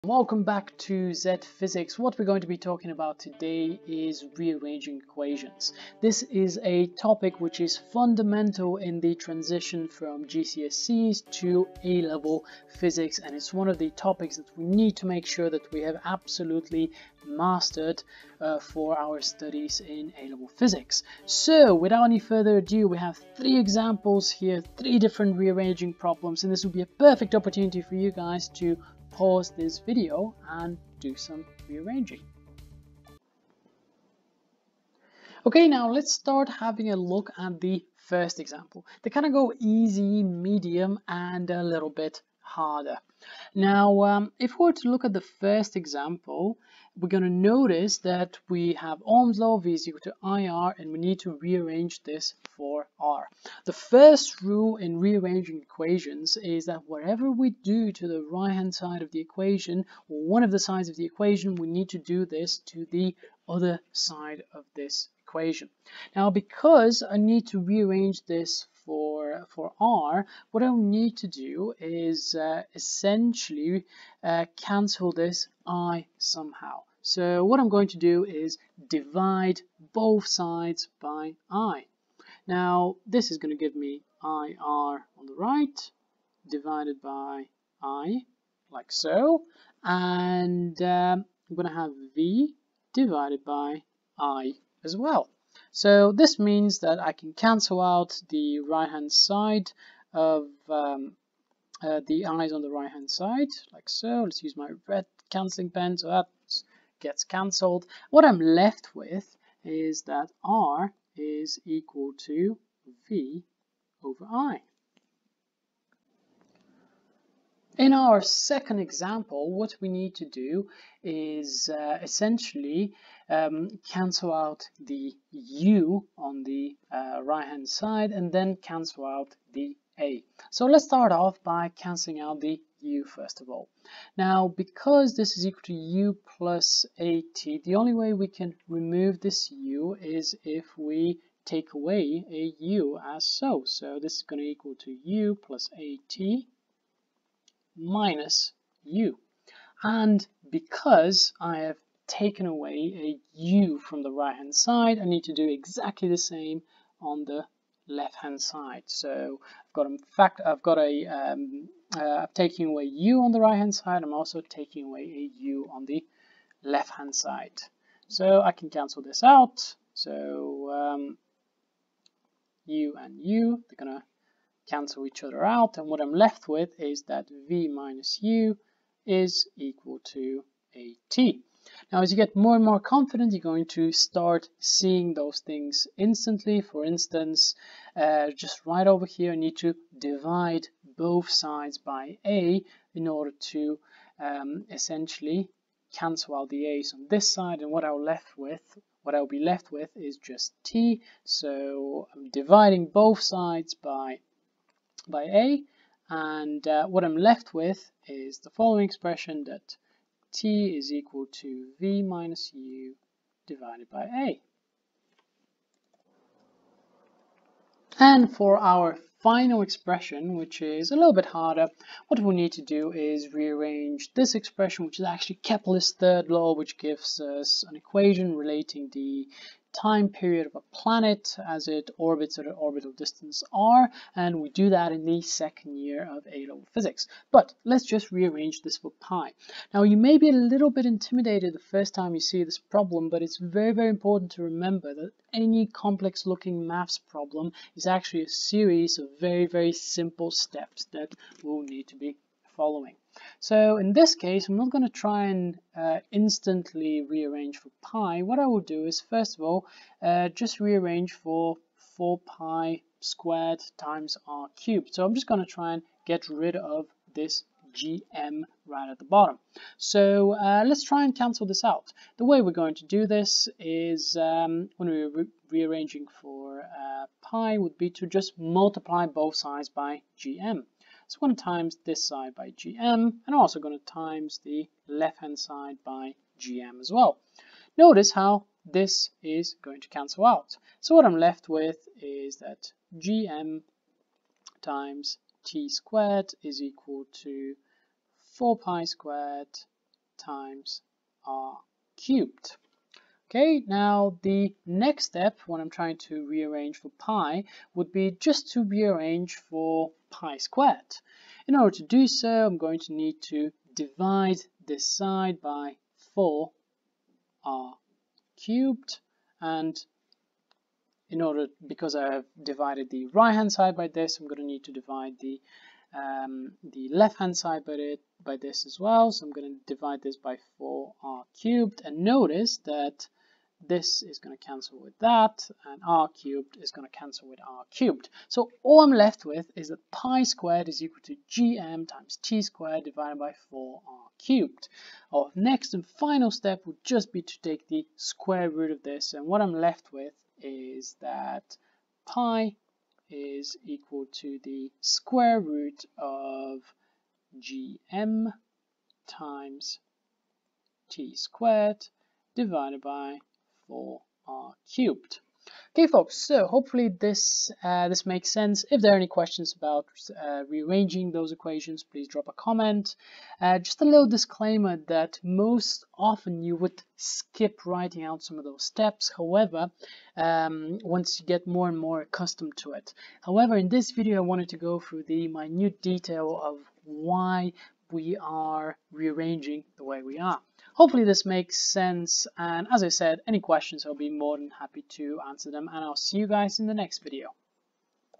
From GCSE to A Level Physics: Rearranging Equations
In the transition from GCSE to A Level Physics, mastering rearranging equations is essential.
Use this video to practice by pausing the video, rearranging and then checking with the answers.